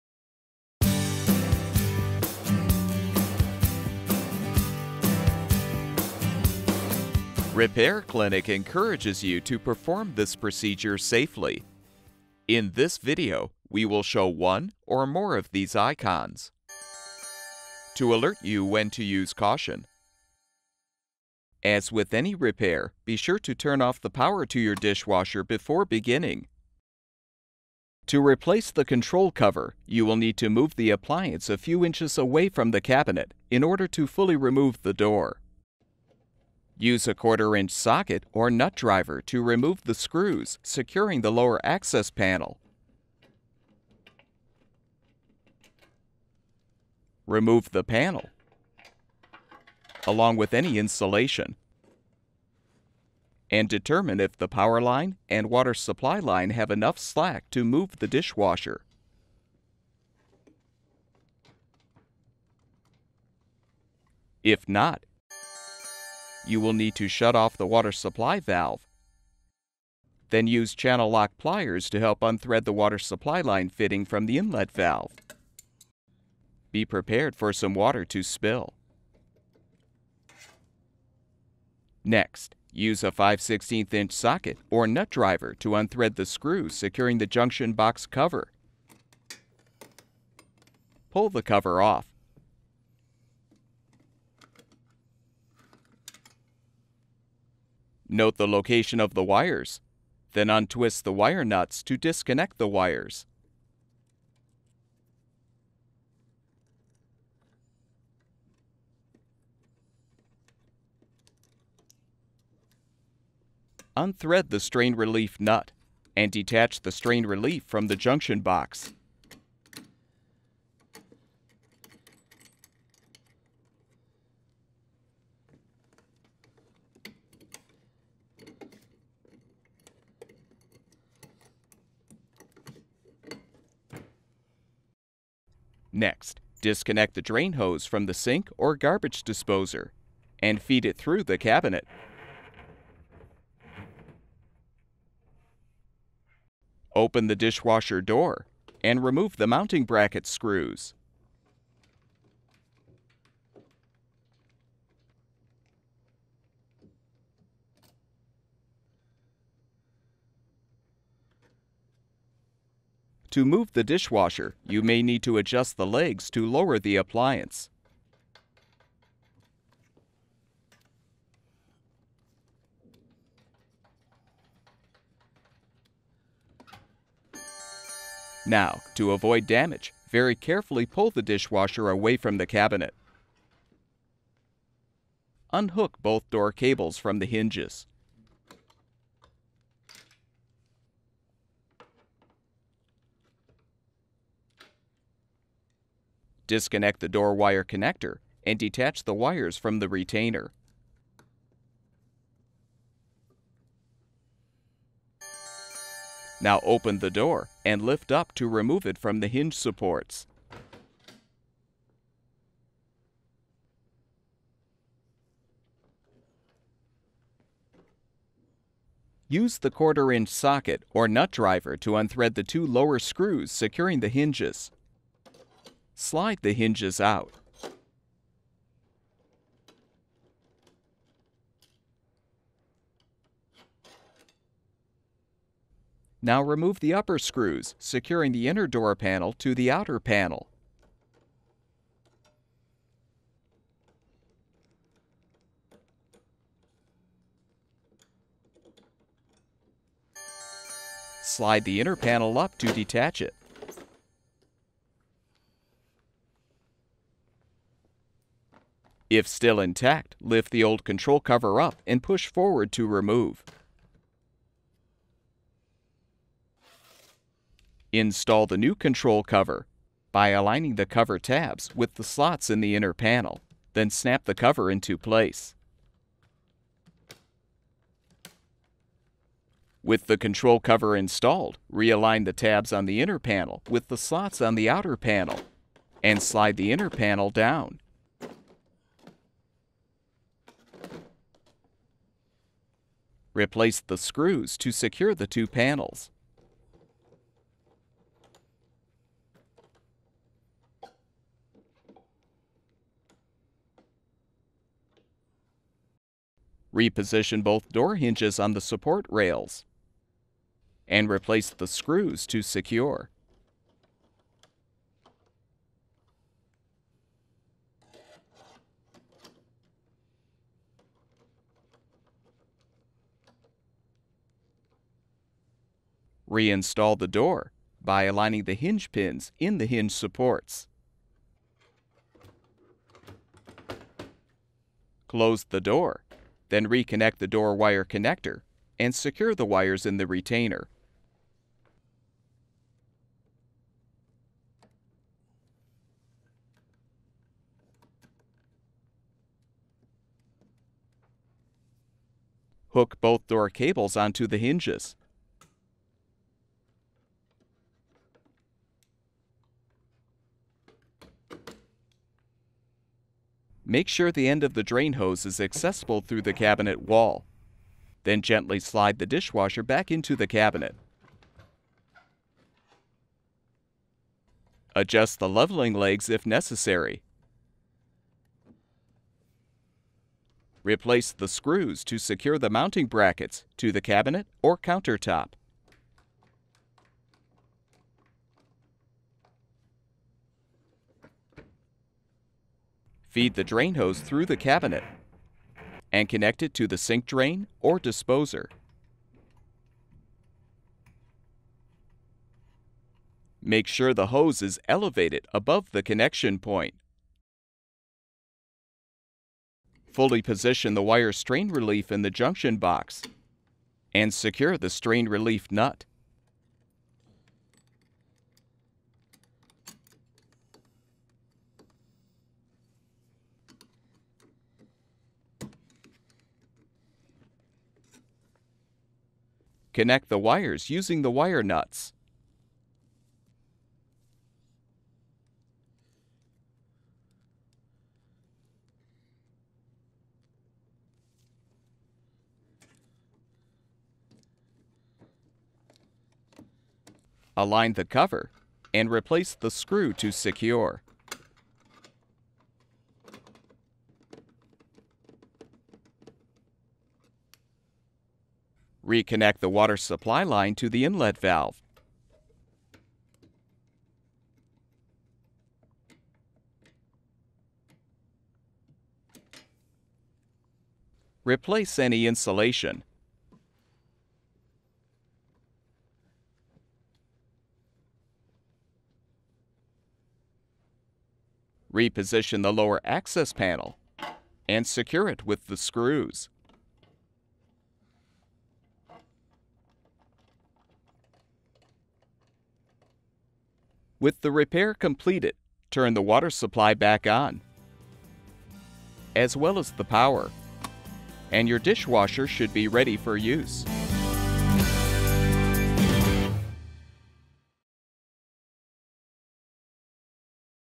GE Dishwasher Control Cover Replacement
This video provides step-by-step instructions for replacing the control cover on GE dishwashers. The most common reason for replacing the control cover is if it is damaged or broken.

Additional dishwasher repair, troubleshooting tips, help with finding your model number, and part replacement videos

All of the information in this control cover replacement video is applicable to the following brands: GE

Tools used: 1/4” socket or nut driver, channel-lock pliers, 5/16” socket or nut driver